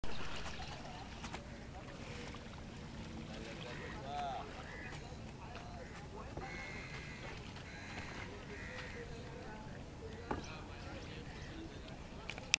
03ganges5.AVI
Boat trip on the Ganges, at sunrise.
en bateau sur le Ganges au lever du soleil